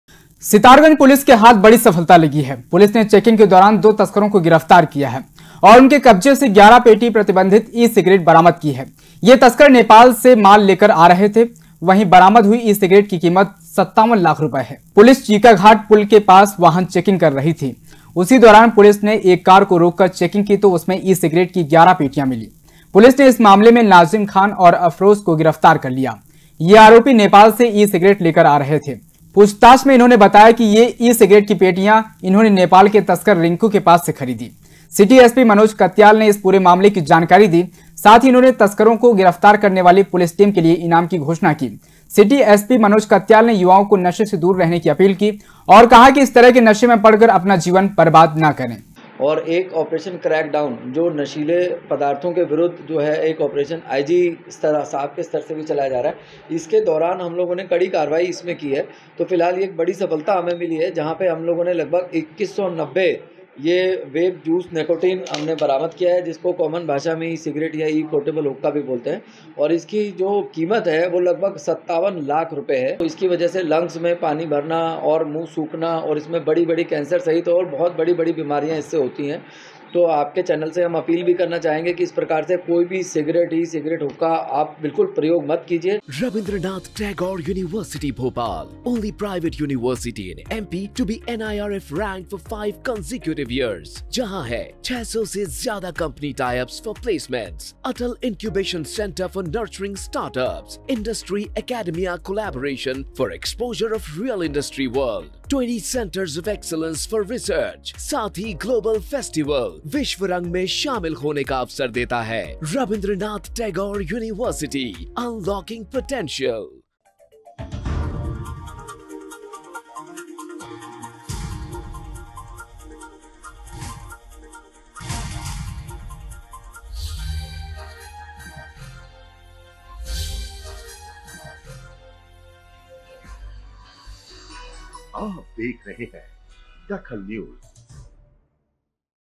DAKHAL NEWS सितारगंज पुलिस के हाथ लगी बड़ी सफलता
सितारगंज पुलिस के हाथ लगी बड़ी सफलता
पुलिस टीम ने दो तस्करों को किया गिरफ्तार
तस्करों के पास से 11 पेटी ई-सिगरेट जब्त की